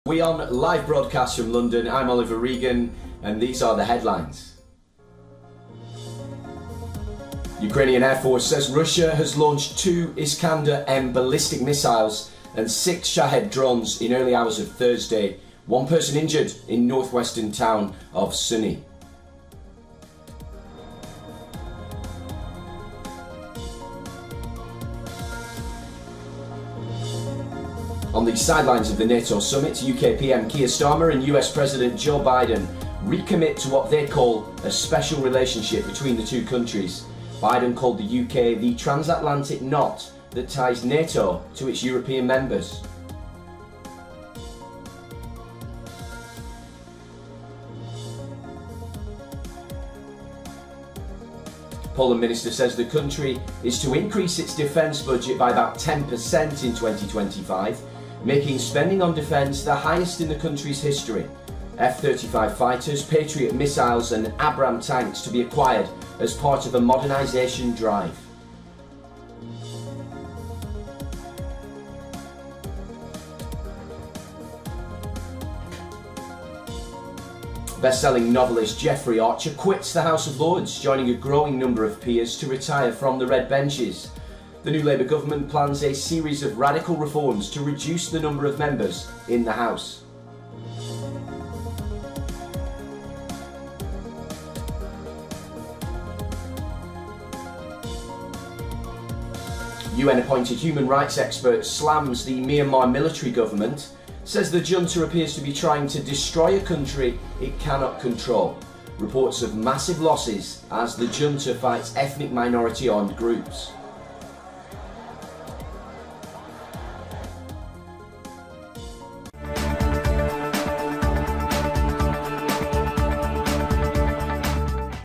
Jeffrey Archer quits House of Lords | Poland defence budget to rise 10% | WION Headlines
Headlines of the hour:

- Russia continues Ukraine aerial assualt
- US, UK recommit to 'special relationship'
- Poland defence budget to rise 10%
- Jeffrey Archer quits House of Lords